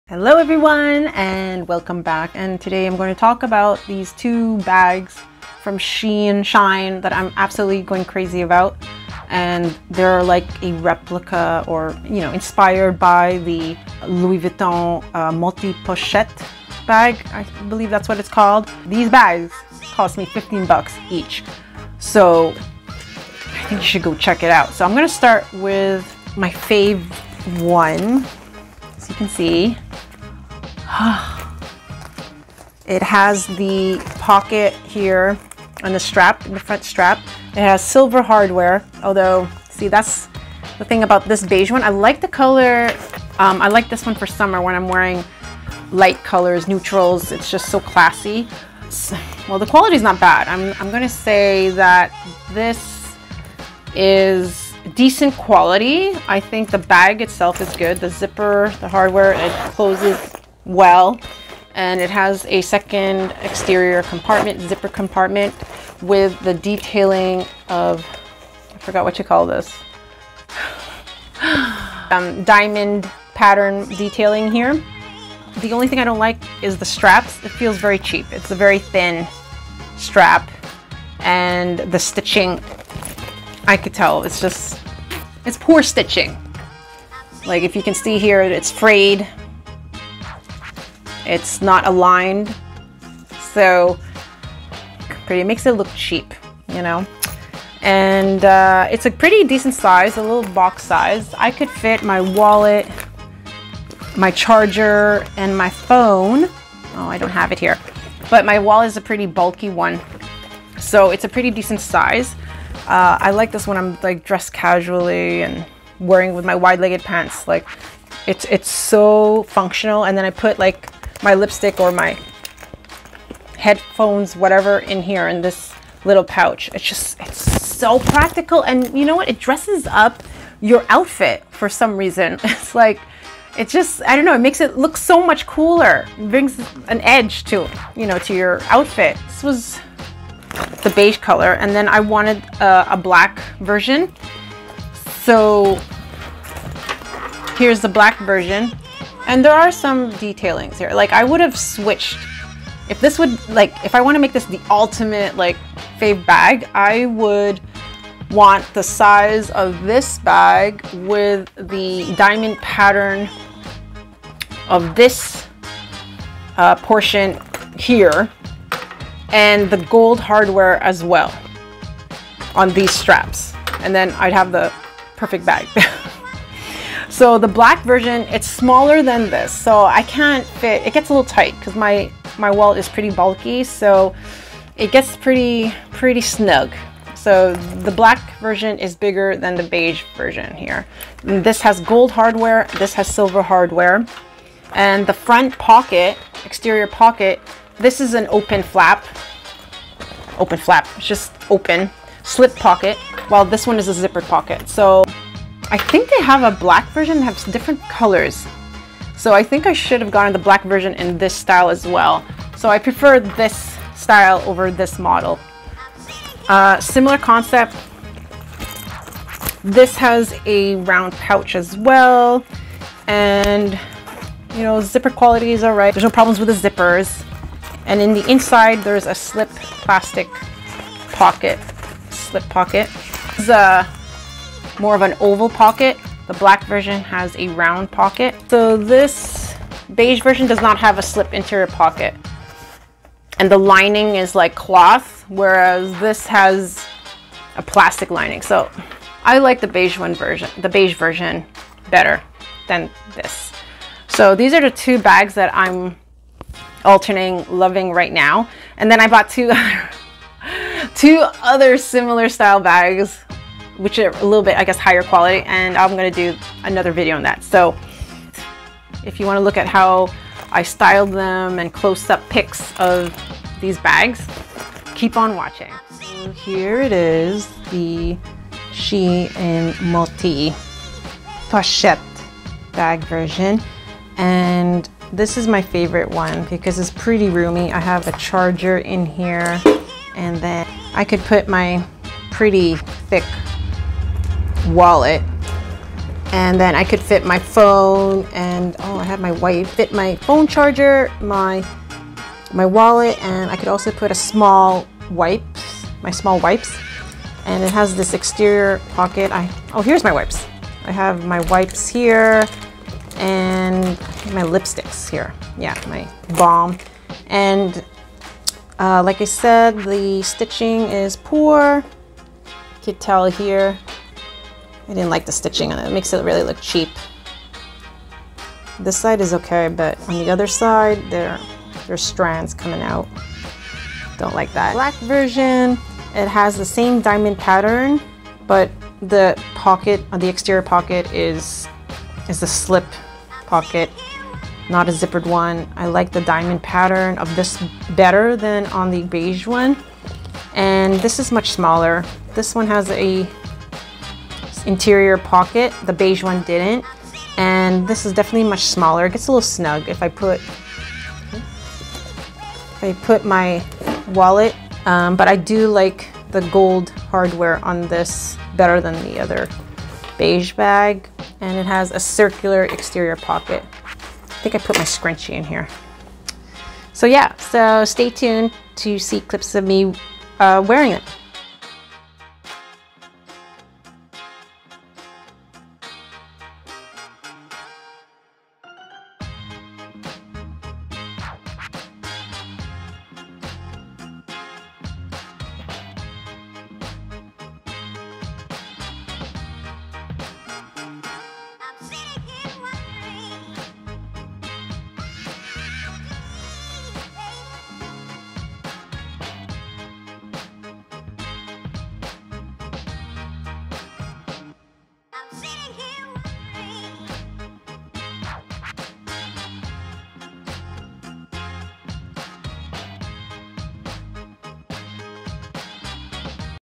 SHEIN Multi Pouch Crossbody Bag Review
Hi Friends,
I'm really loving these bags from SHEIN. They're functional and cool! Not to mention, affordable. If you're looking for a decent quality bag on trend that will add some ooomph , check these out.

Beige BAg:
 EMERY ROSE Minimalist Quilted Square Bag With Small Pouch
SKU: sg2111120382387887

Black bag:
Mini Quilted Square Bag With Coin Case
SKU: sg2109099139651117